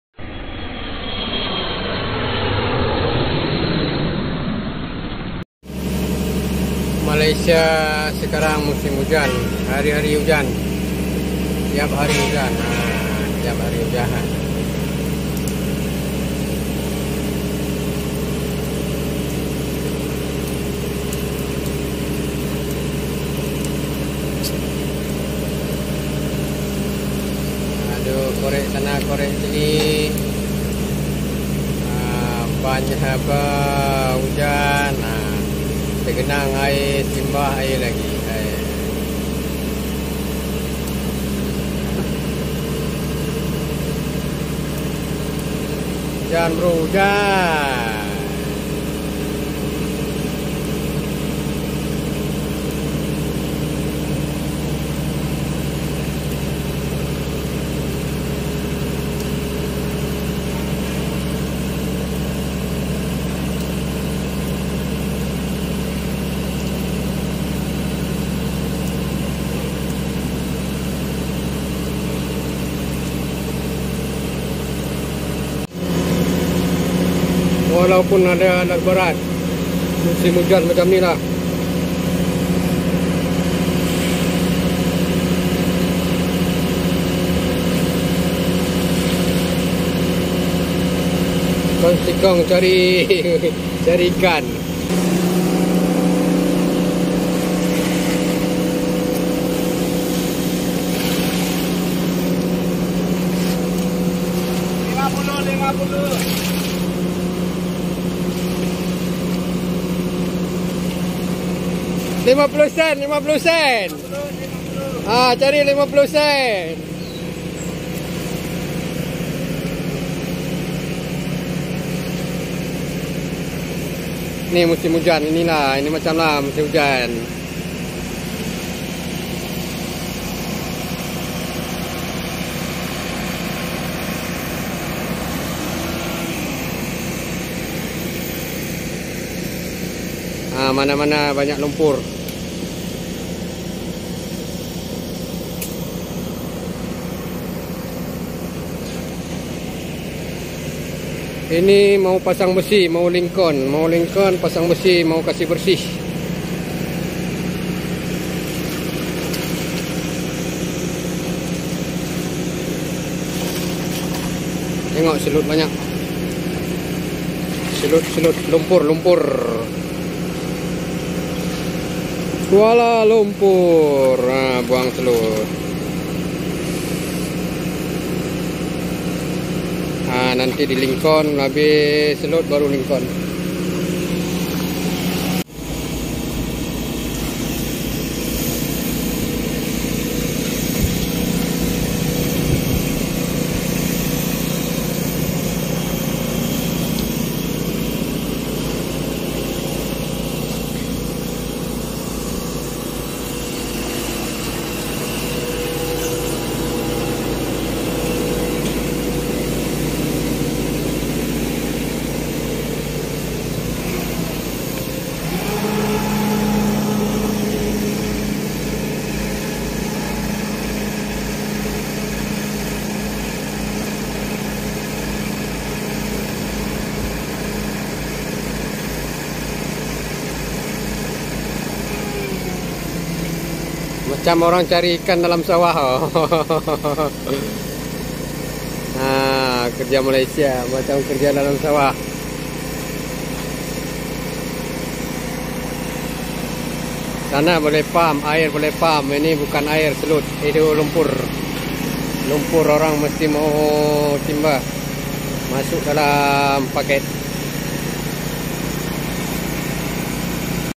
HUJAN DI ECO MAJESTIC || TKI MALAYSIA ~Bailuk.
Malaysia sekarang musim hujan,tiap hari hujan.